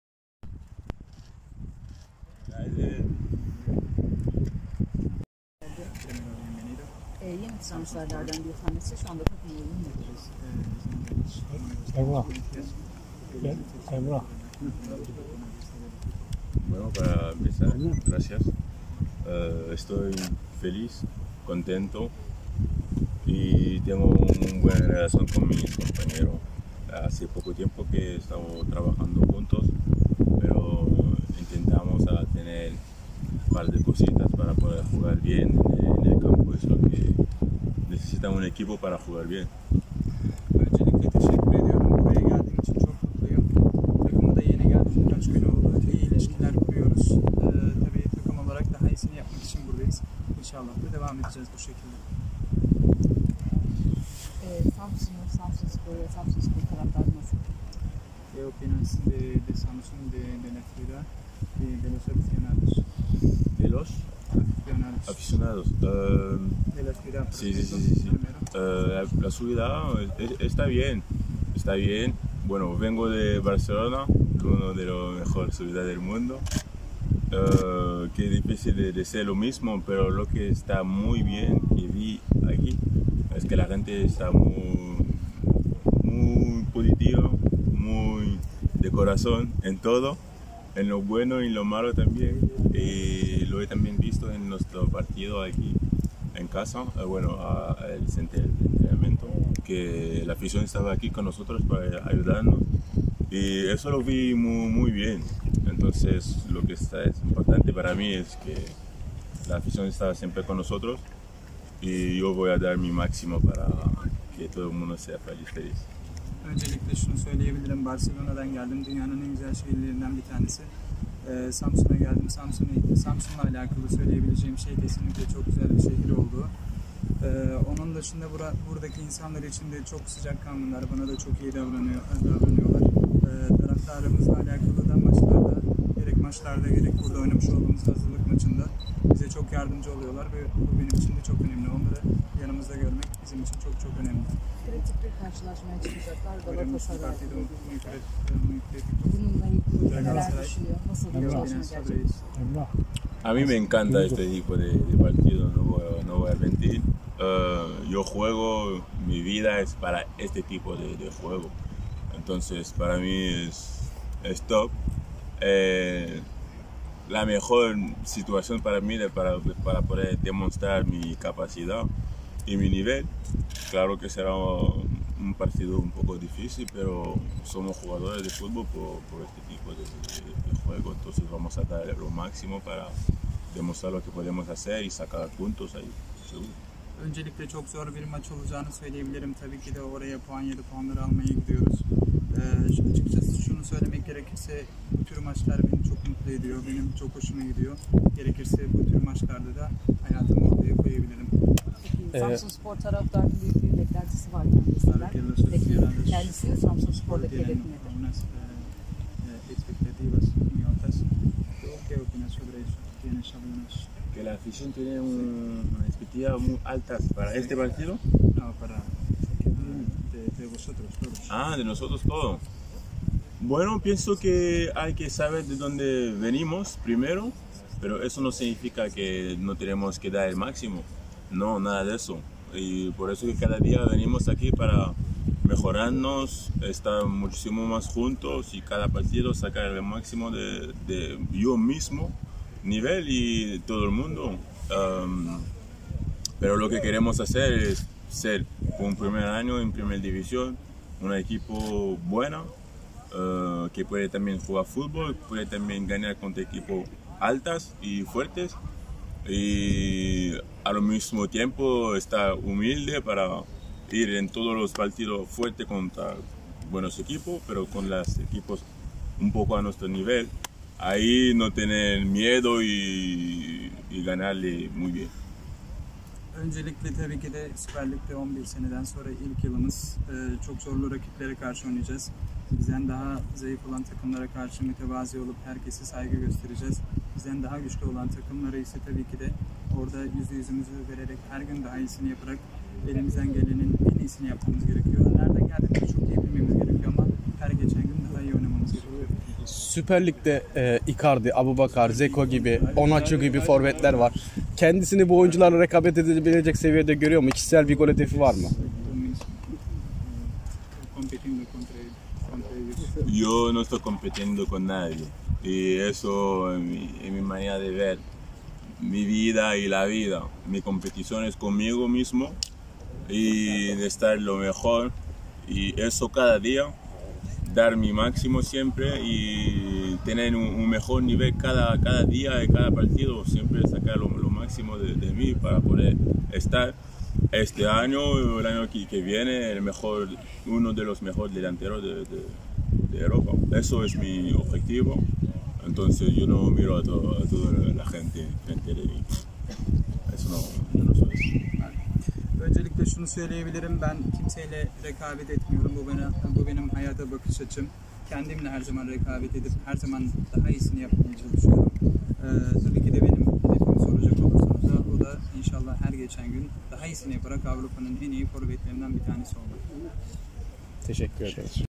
Samsunspor'un Yeni Transferlerinden Landry Dimata İle Röportaj
Samsunspor'un Yeni Transferlerinden Landry Dimata antrenman öncesi basın mensuplarının sorularını cevapladı.